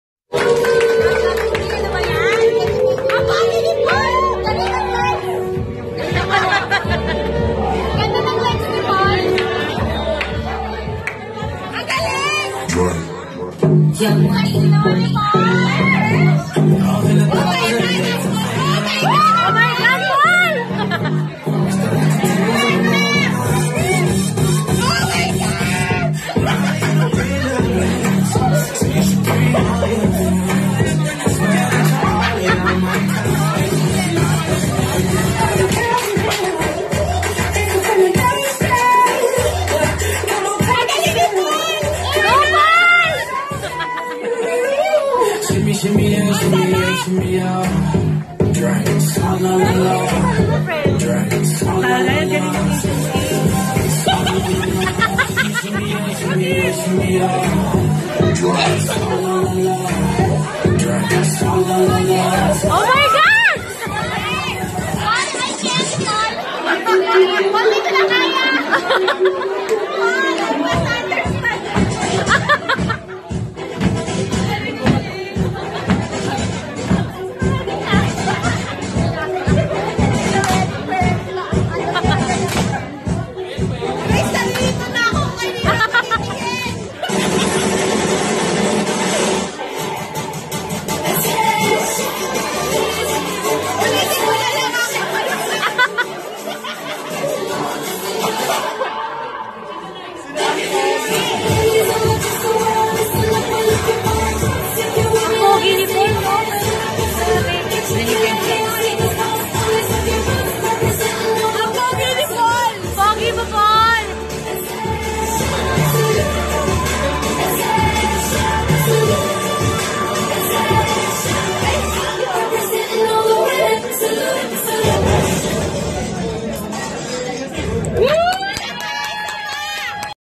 PPD Academy Opening Dance Number
PPD 4th Year Anniversary Celebration opening number by PPD Academy

Video courtesy: Cha Nallano